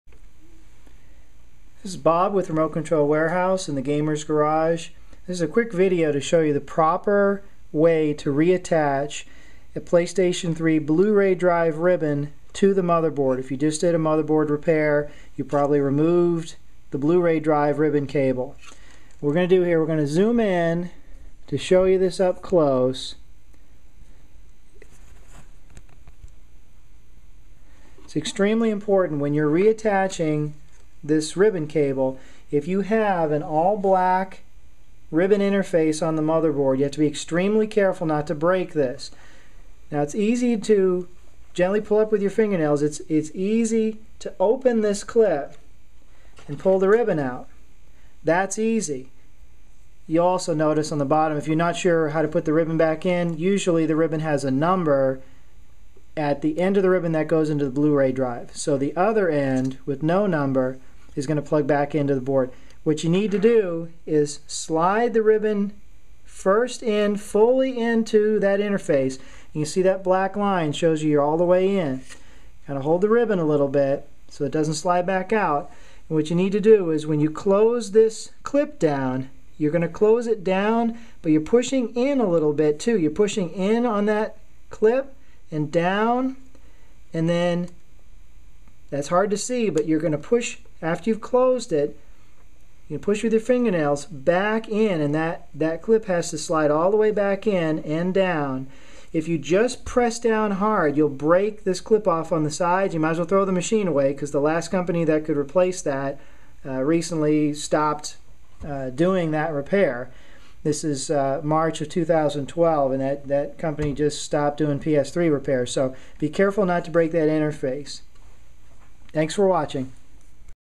How to Attach a PS3 Bluray Drive Ribbon Cable to the Motherboard
Discussion of proper reattachment of a PS3 Bluray drive ribbon cable to the system board. Don't break the interface clip and ruin your Playstation 3. (RemoteControlWarehouse.com Gamer's Garage)